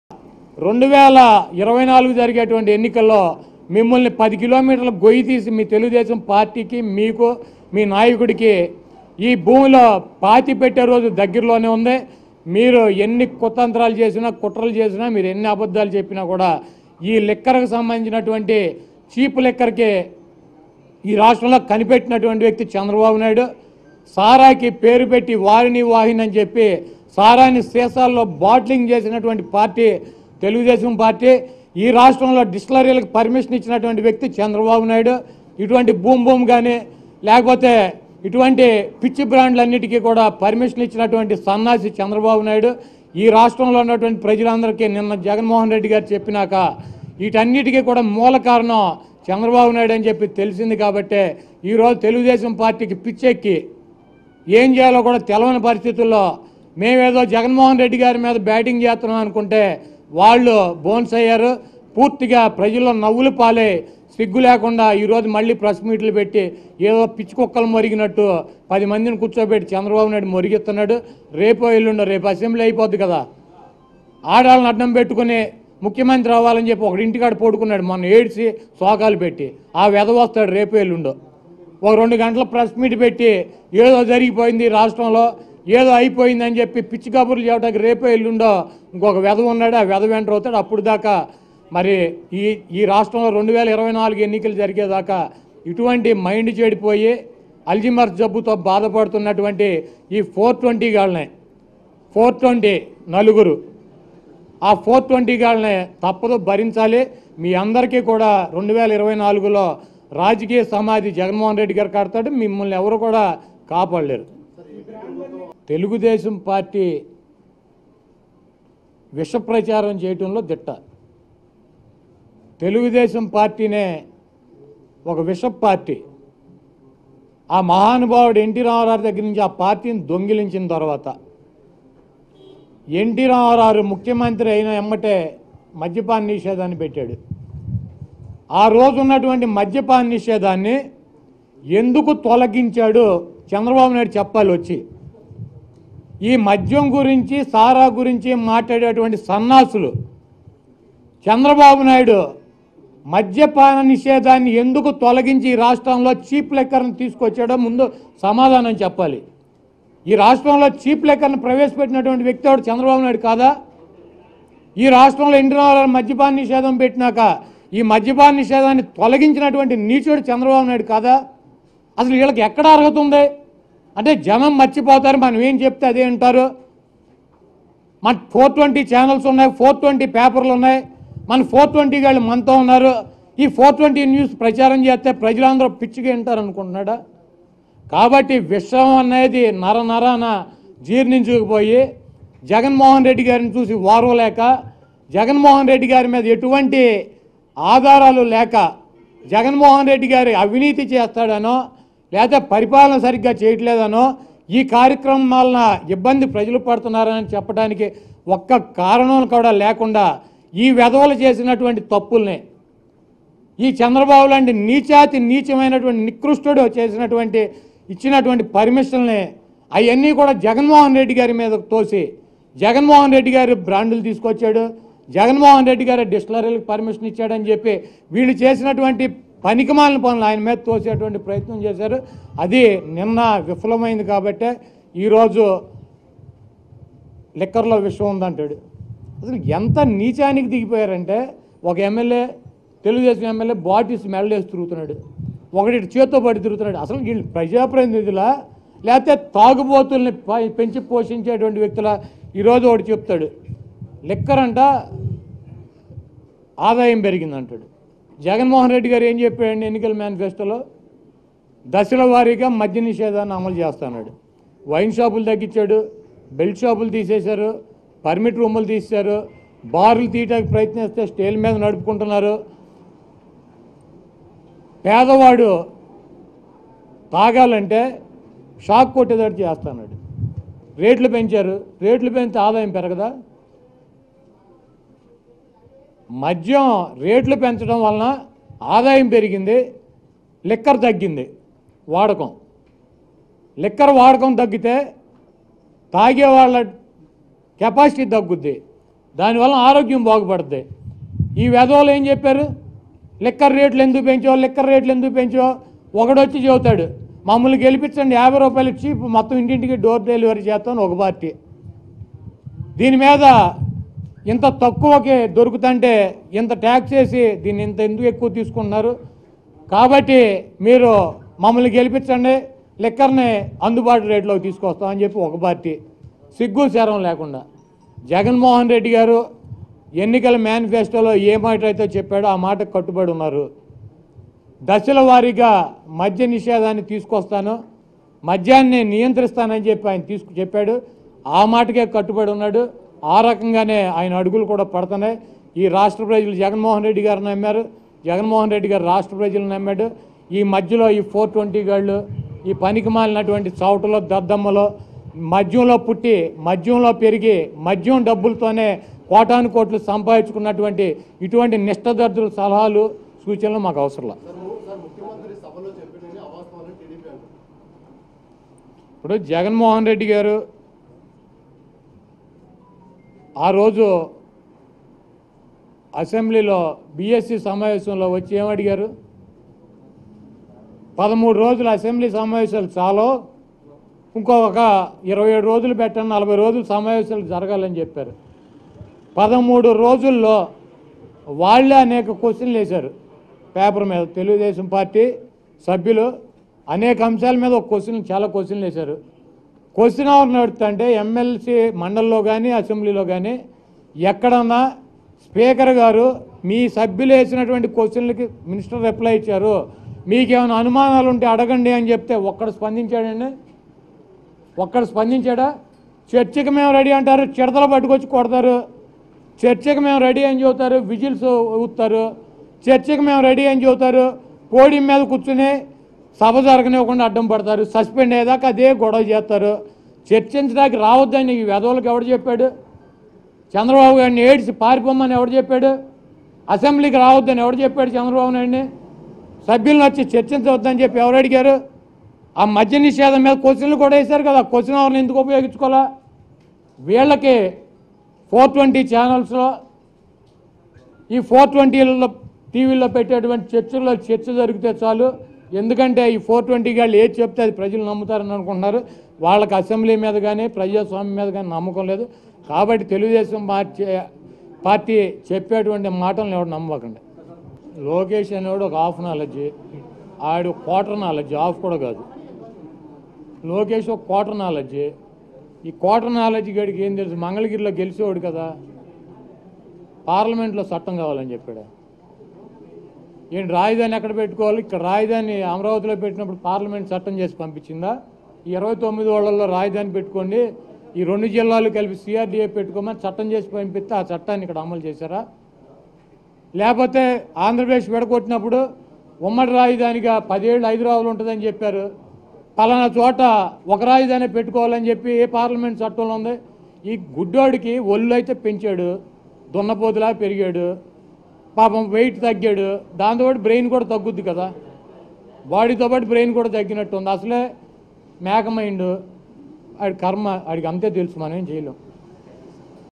రాష్ట్రంలో చీప్ లిక్కర్ ని తెచ్చిందే చంద్రబాబు..2024 లో నీకుంది...||Kodani Nani Fires on Chandrababu
Watch► రాష్ట్రంలో చీప్ లిక్కర్ ని తెచ్చిందే చంద్రబాబు..2024 లో నీకుంది...||Kodani Nani Fires on Chandrababu